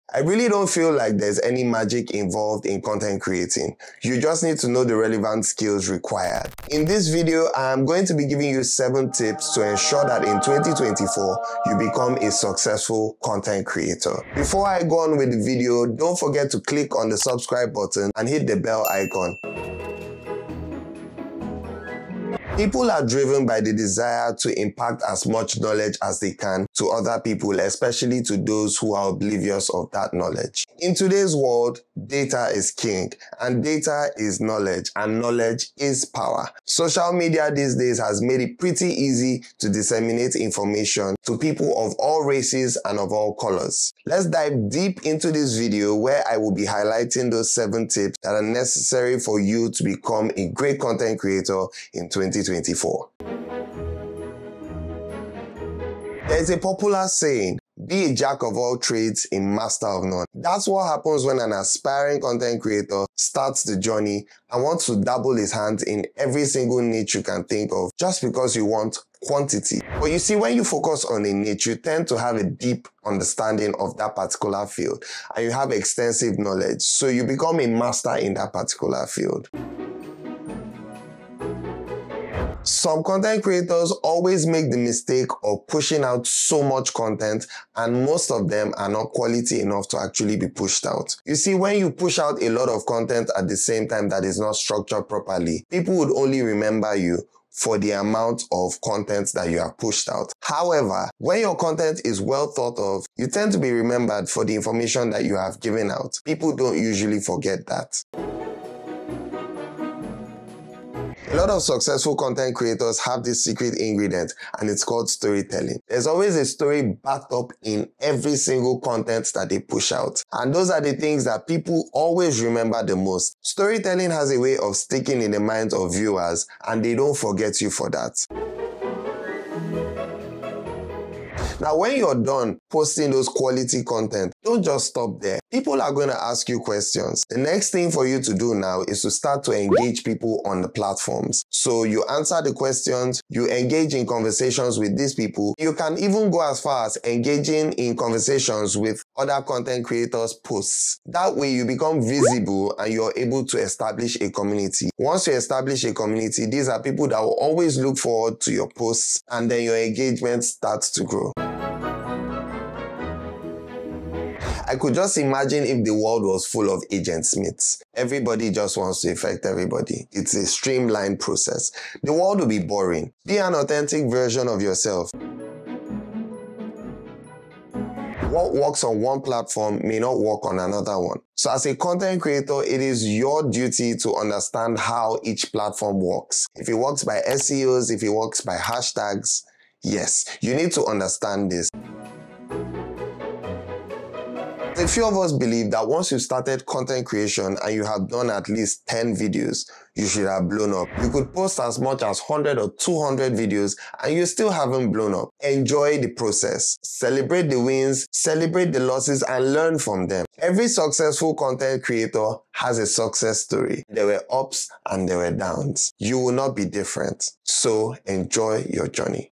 7 Tips To Become A Great Content Creator in 2024 (Beginner's Guide)
Do you want to be a content creator who dominates in 2024? Or are you a beginner content creator? Buckle up, because this video is your roadmap to sure success!

We're dropping 7 SECRET tips that'll transform you from a constantly tagged "newbie YouTuber" to a magnetic content creator.

Own the territory in which you plant your ideas. Own your niche and use it to your advantage. Be a master in the topics that most interest you.
No matter how much you create as a content creator, nothing beats the quality of what you put out on YouTube for consumption. Make sure you prioritize having informative ideas or content.
Children love stories; adults do too. So make it a point of contact. Engage your audience in the art of storytelling and watch your fanbase rise quickly. Always have a hook at the beginning to keep your audience engaged.
Don't just post and watch Netflix and chill..make sure you can engage with your audience when questions are asked. Also, be ready to throw more insight into the topic at hand. Make sure you encourage engaging in other posts on your timeline.
Be as authentic as you can be. Yes, there are many content creatives out there but just one of you. Never be in the act of duplicating another person's style. Rather emulate yours and bring your own spice to the social media space. Be original.
There is nothing as bad as knowing how much effort you have put into content made and it doesn't do so well. Learn keywords that'll make your video explode in search results. Don't forget to use hashtags relevant to your topic.
If you have posted a couple ten videos and you haven't touched the stars, celebrate lifting off the floor. Nothing good comes easy. It is all a process that should be celebrated whether it is a win or loss. Now you can sit and consume content from Netflix while you watch your Youtube channel grow.